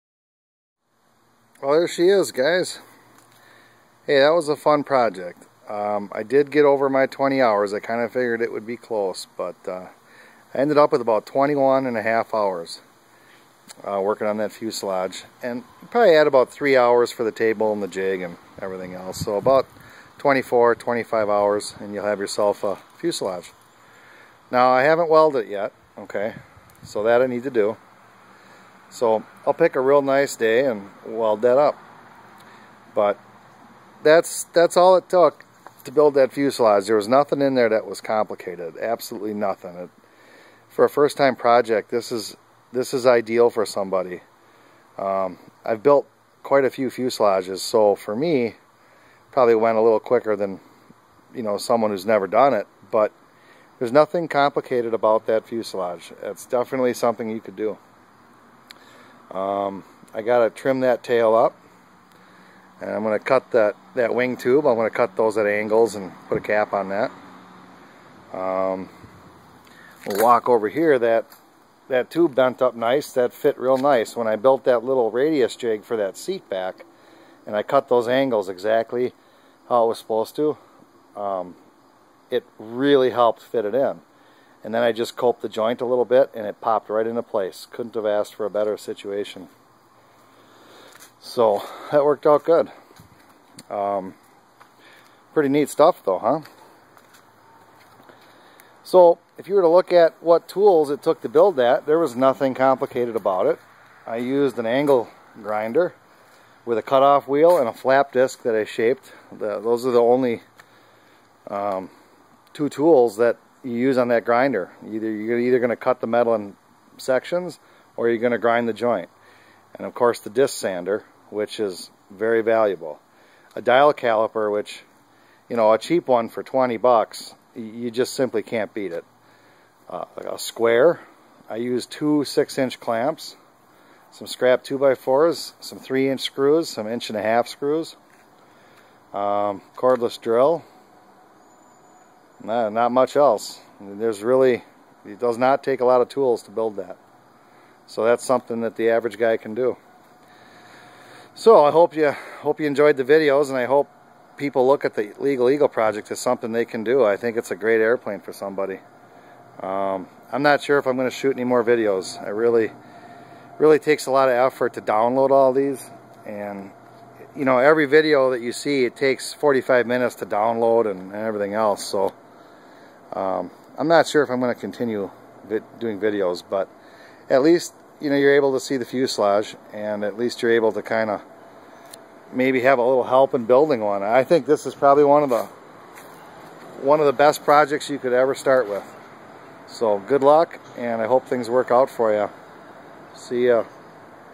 Legal Eagle Ultralight construction video #65
Final fuselage video for the Legal Eagle Ultralight Airplane. Building the fuselage for this aircraft was a lot of fun and quite simple. I hope you had fun watching the videos and I hope it gives you the confidence to build one for yourself. The total cost of the materials to build the fuselage up to this point was $480 as of August 2012 and it took me 21-1/2 hours. Not bad all things considered. Good luck and take your time! Most importantly. have fun!